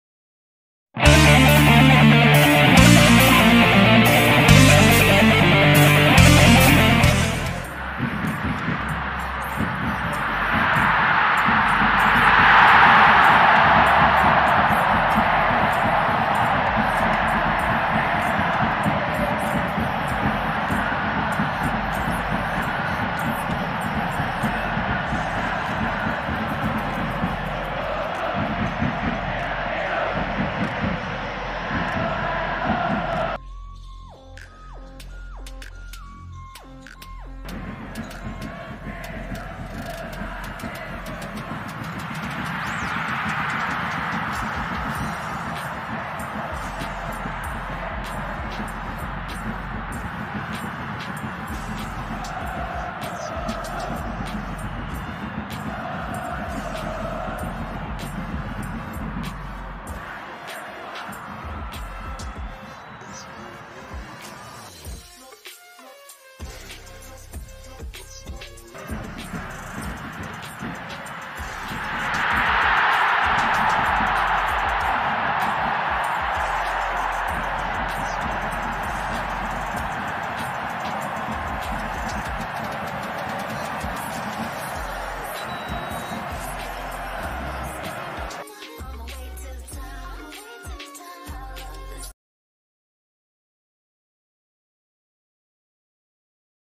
Highlight JDT vs UiTM CIMB Liga Super
JDT menewaskan UiTM 2-1